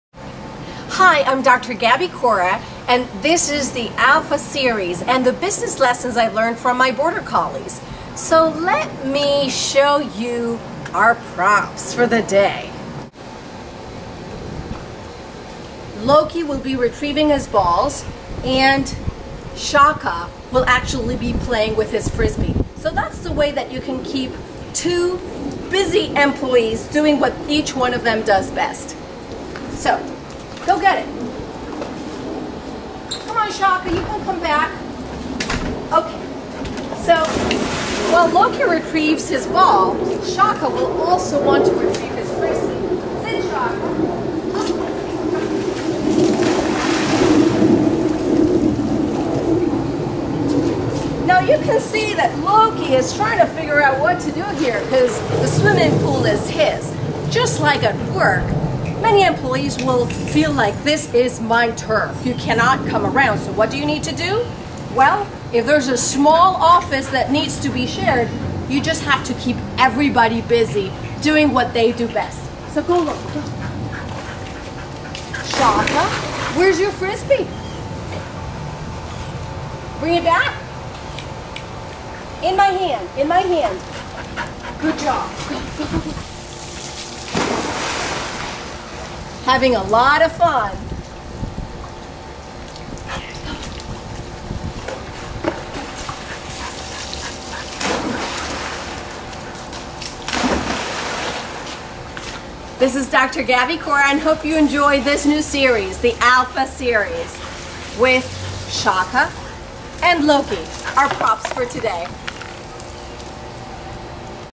Business Lessons: Alphas at Work
In this video, Dr. Gaby Cora introduces the Alpha at Work with her assistants, Loki and Shaka. Anxious employees with a lot of energy need to be kept busy with direct orders and tasks to keep them on track. Alphas need to be kept busy with the thing that they love doing most, allowing the others around them to work well.